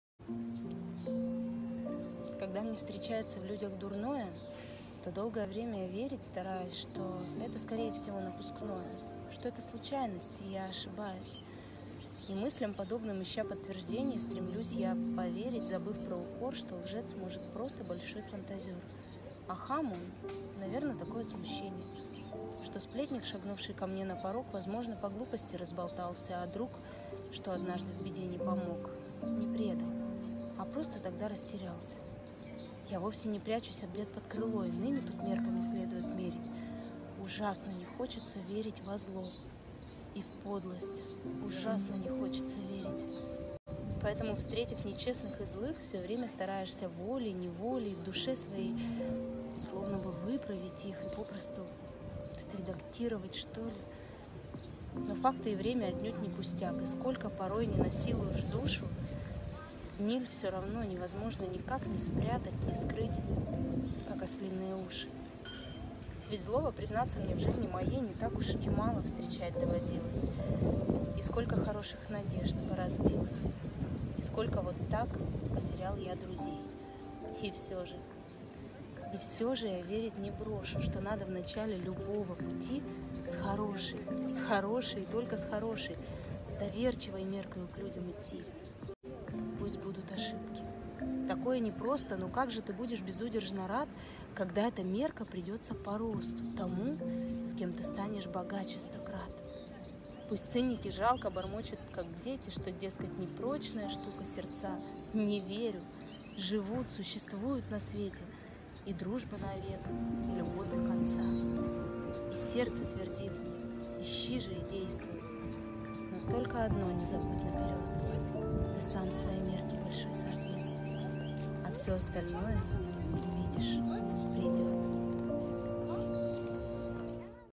Когда мне встречается в людях дурное I Эдуард Асадов
Читает egzaitceva89 (instagram @egzaitceva89)